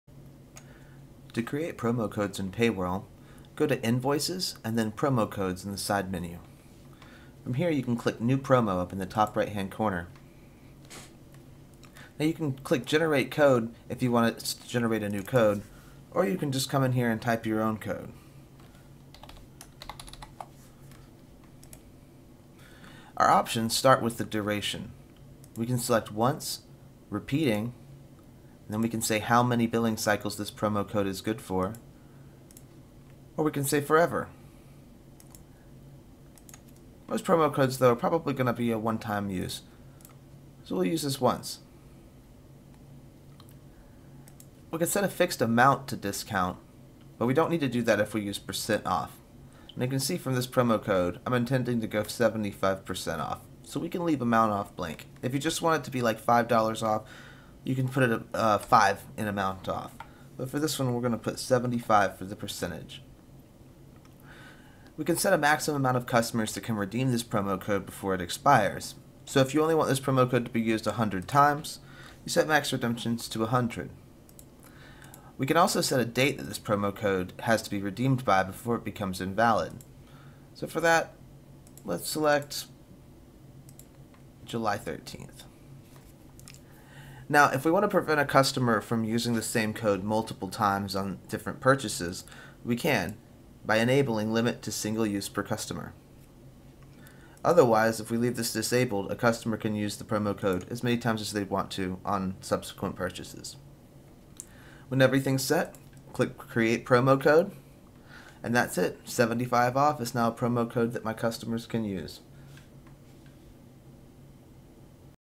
Create Promo Codes with PayWhirl
Create promo codes that you can give your customers to offer a variety of discounts on pricing for your subscription business.